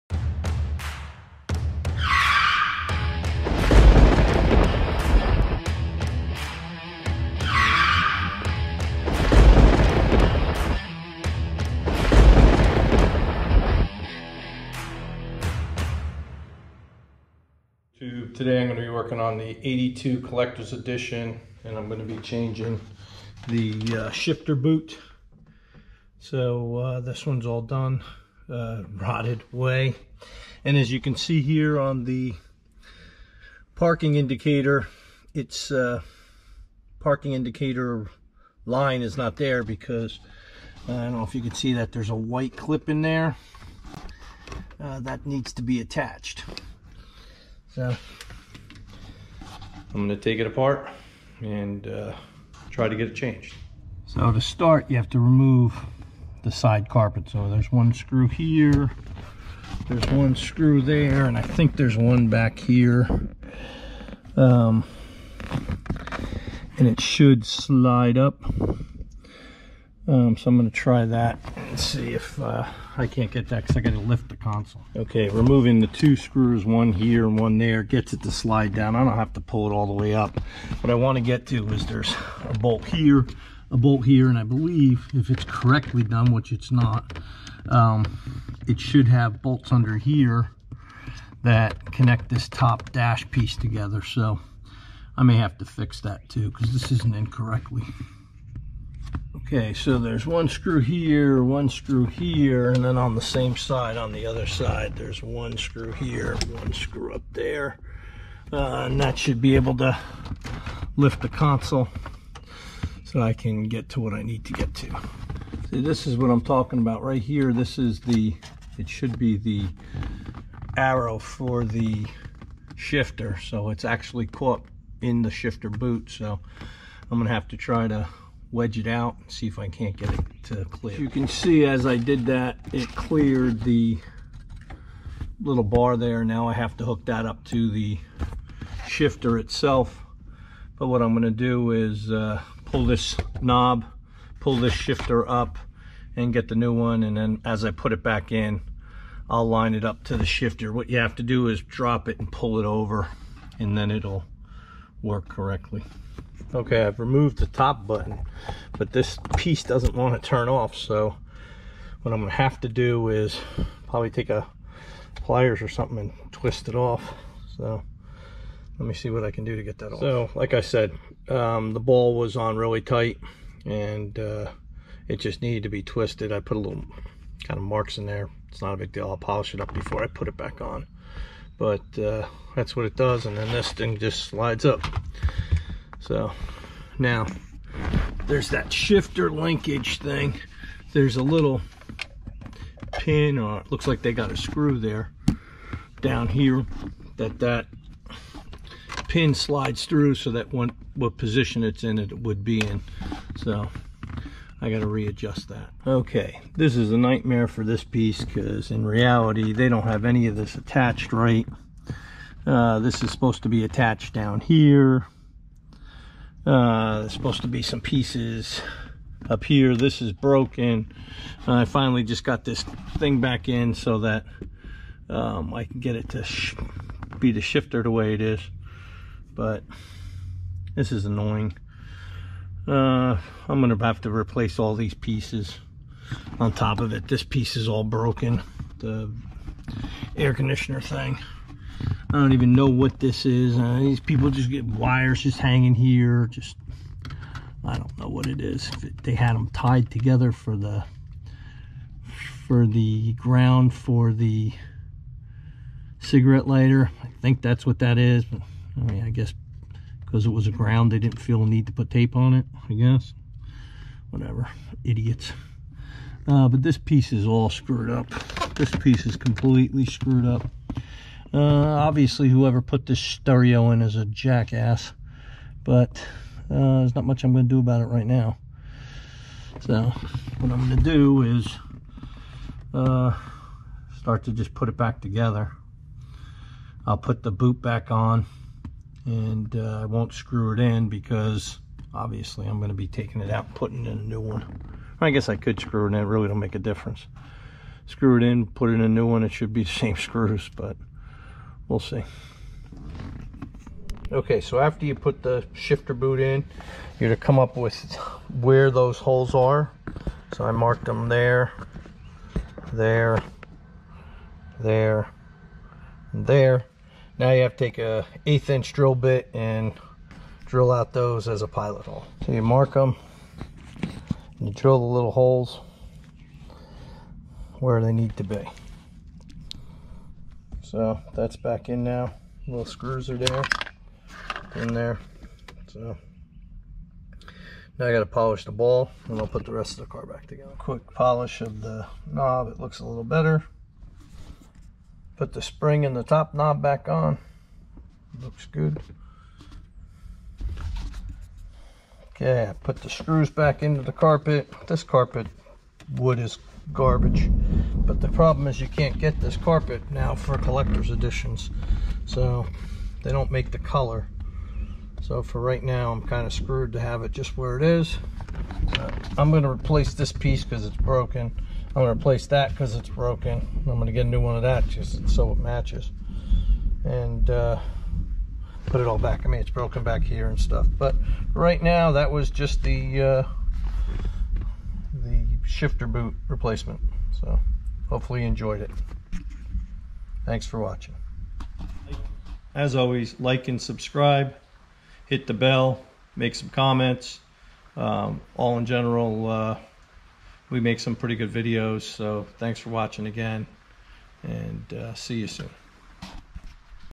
TIPS AND TRICKS C3 for shifter boot replacement on C3 Collectors Edition Corvette
changing shifter boot on a C3 Corvette